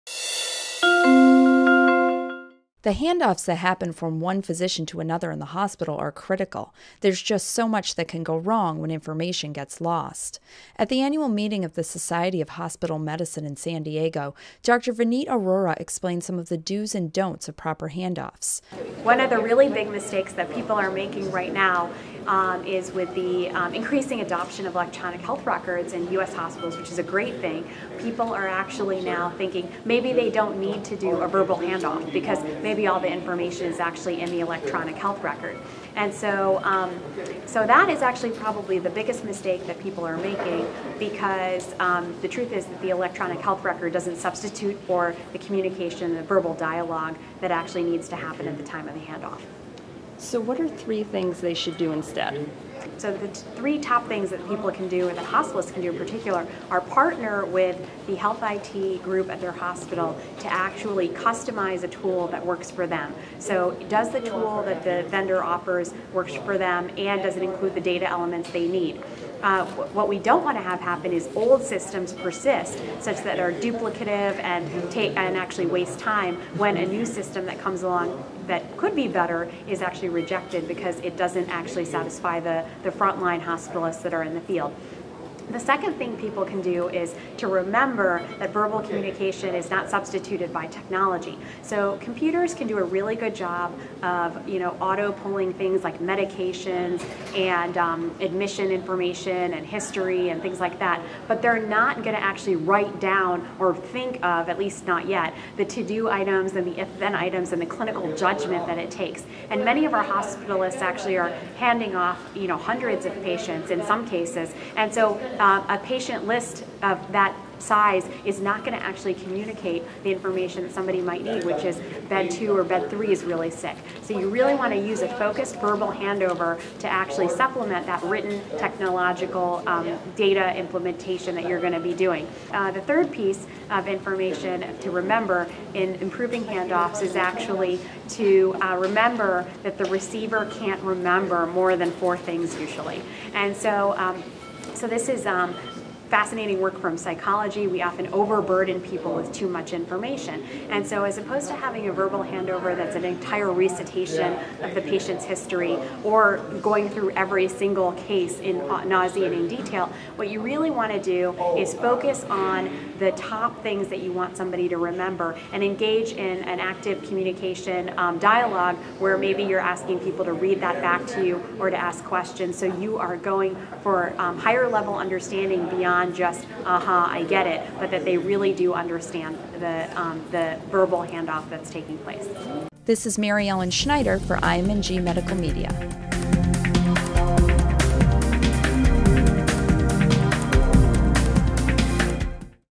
Do's and Don'ts of Patient Handoffs
Hospitalists may be tempted to rely on their electronic health record systems to do all the work on patient handoffs, but that probably won't turn out very well. Instead, Dr. Vineet Arora offers three strategies to improve patient handoffs.